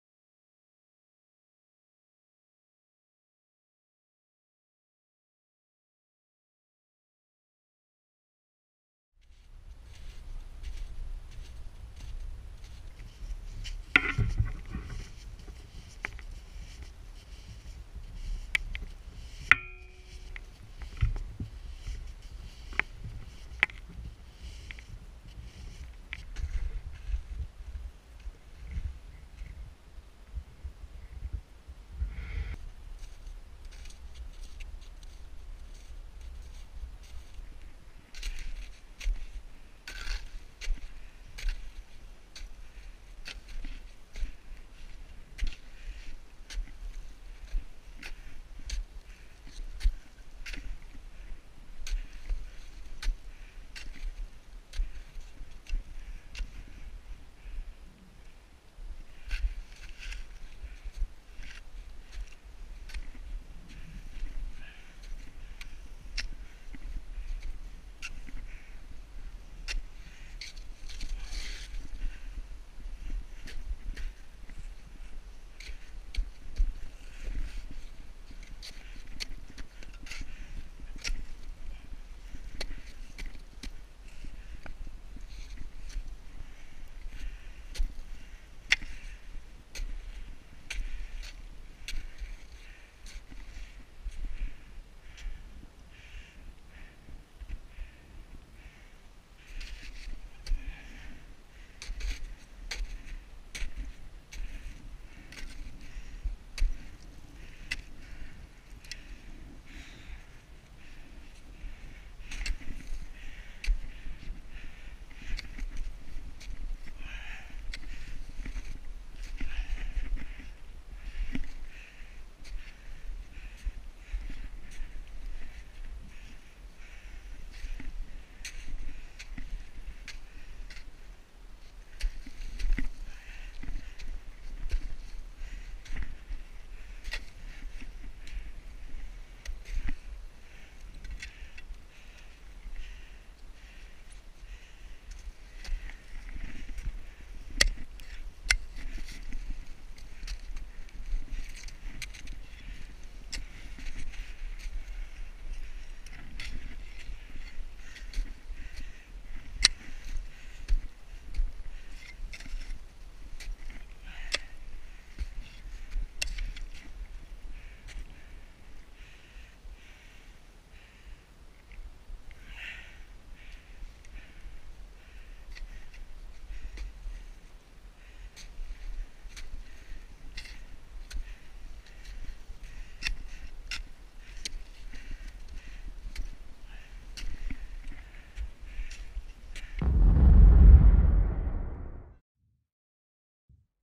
GoPro Winter Trek of Scafell Pike Jan 16
GoPro Winter accent of Scafell Pike from Seathwaite.